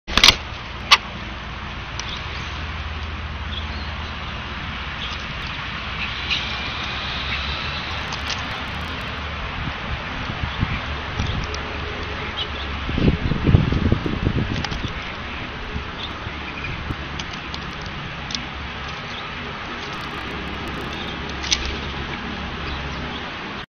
Red-bellied Woodpecker
We call him Peanut, he is the resident male Red-bellied Woodpecker. Proudly announcing his presence before he picks some goodies to go.